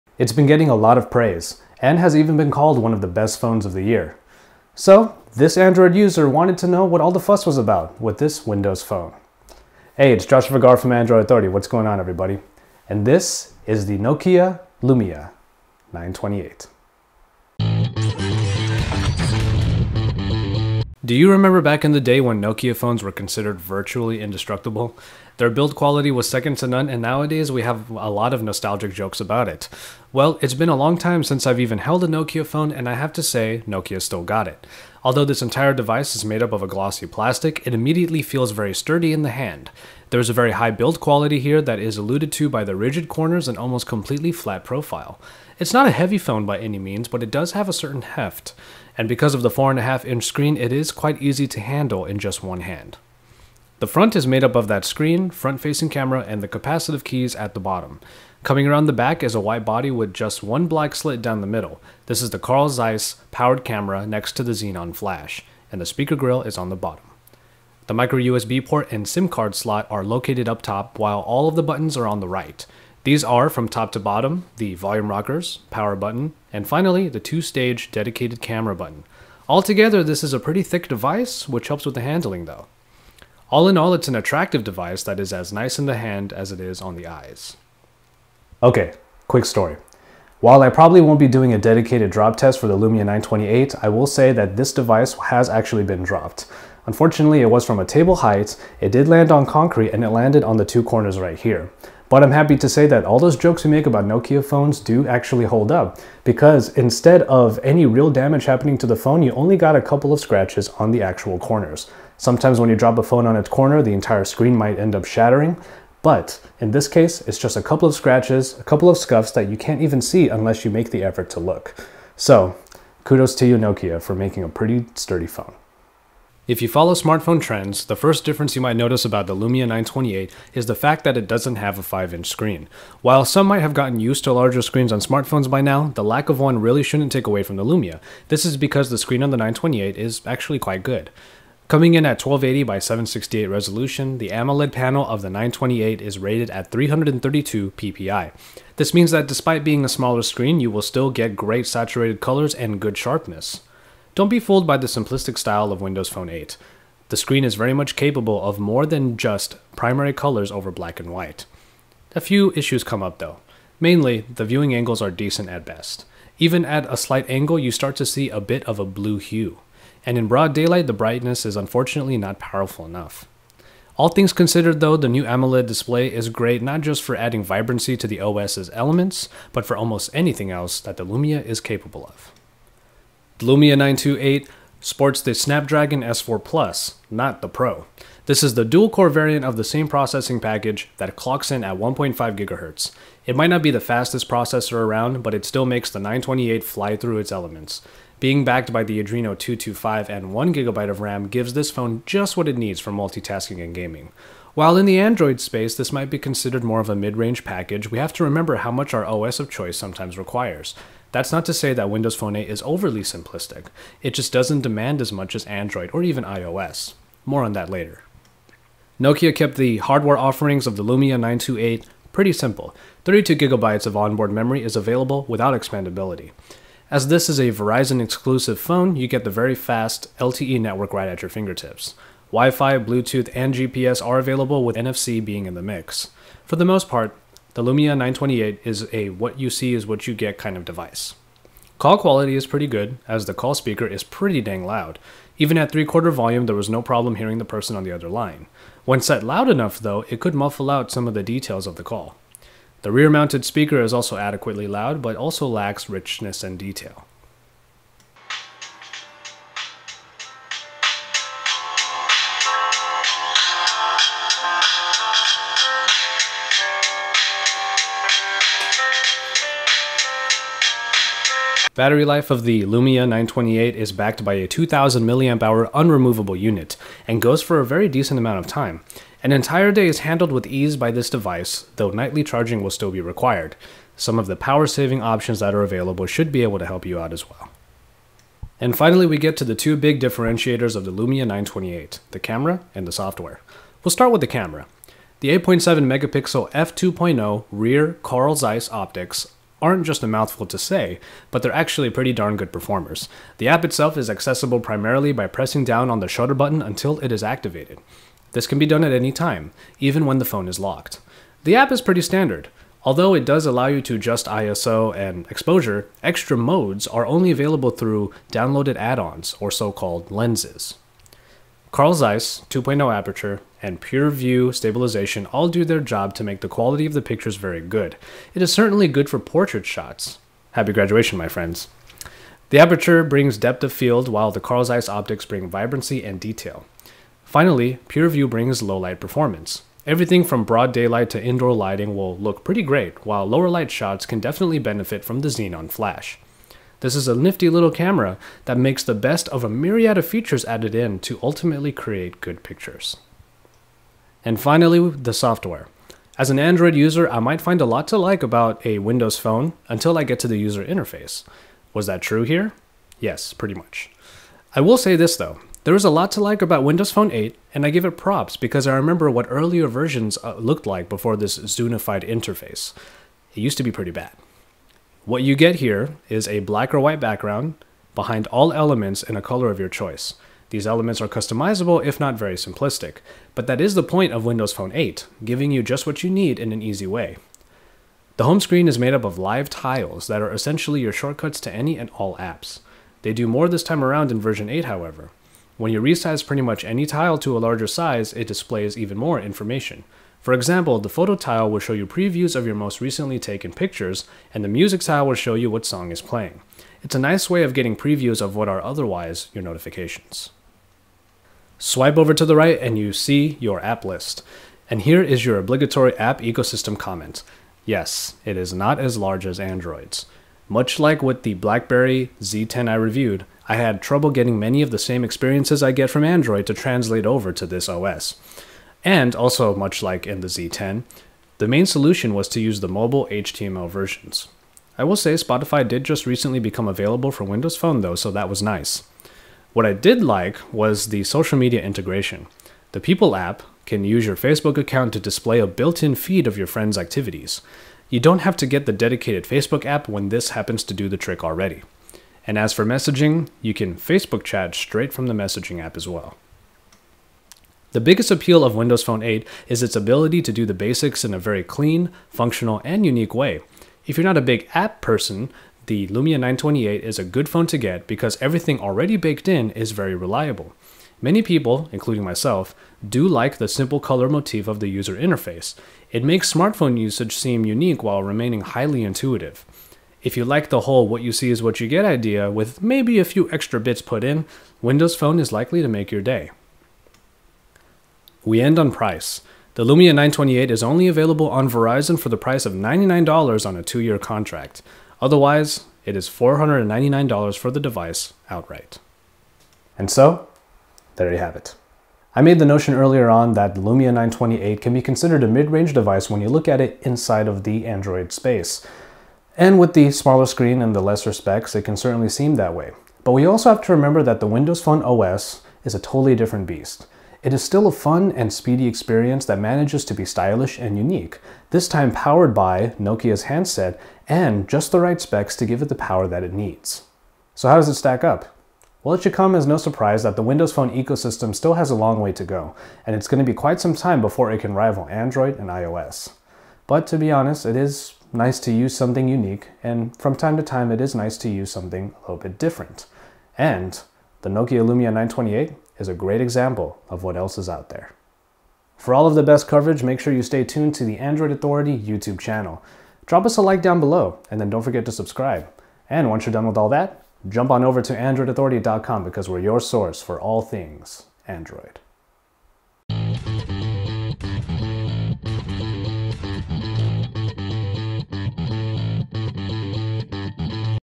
Nokia Lumia 928 Review - Confessions of an Android User
It's been called one of the best phones of the year, so obviously we had to know what all the fuss was about. Josh takes an Android user's perspective on the Windows Phone 8-powered Nokia Lumia 928.